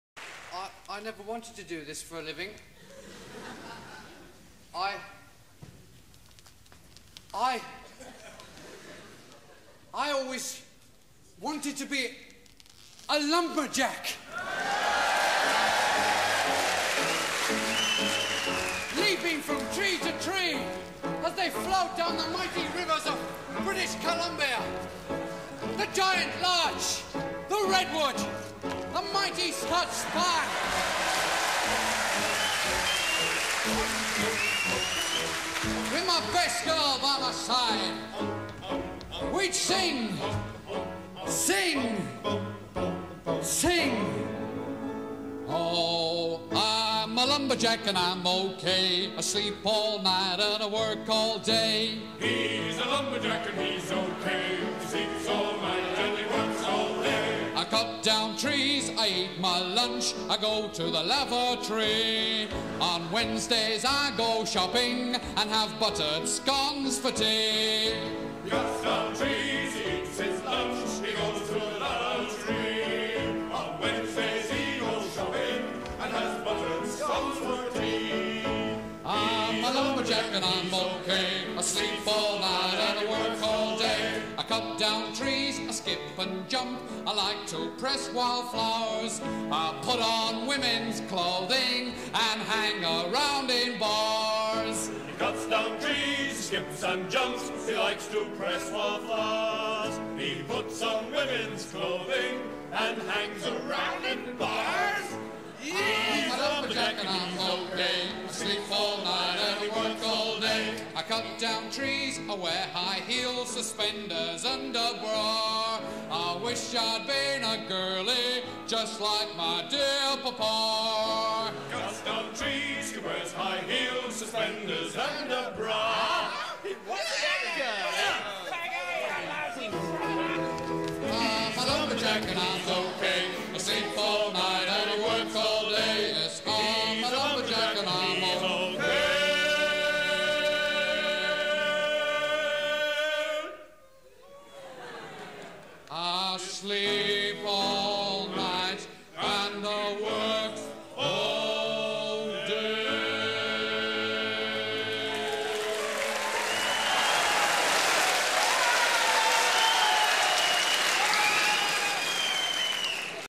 The classic do-gooder anthem song.wmv
It may be a classic to many but try this version but remember to have the sound down just enough to get the tempo. Its all about the lyrics, you'll be singing along in no time, especially the chorus.